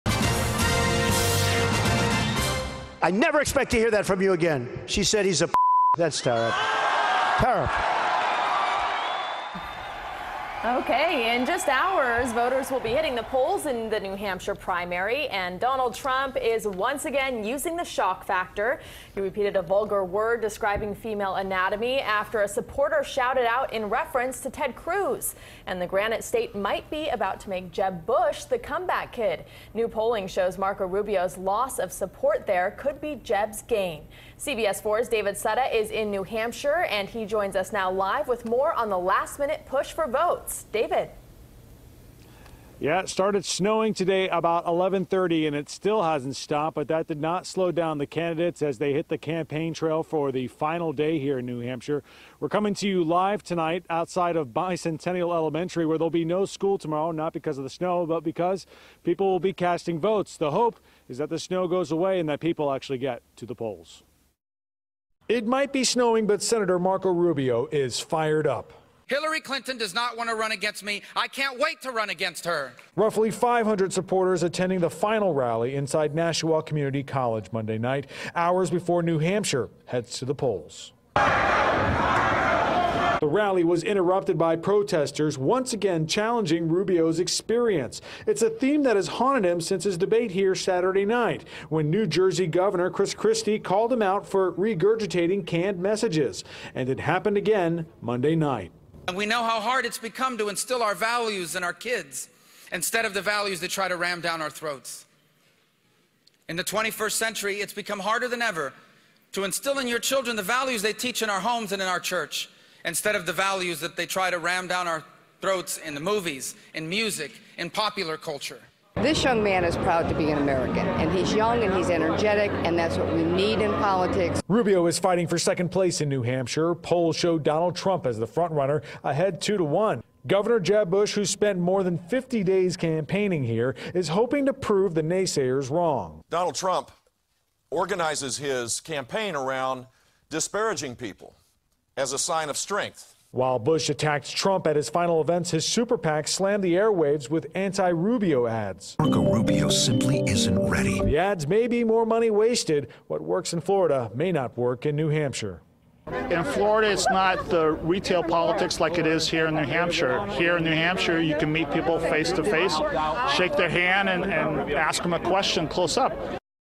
As Snow Falls Over New Hampshire, Heat Turned Up On Primary
Watch David Sutta’s Report for CBS Miami.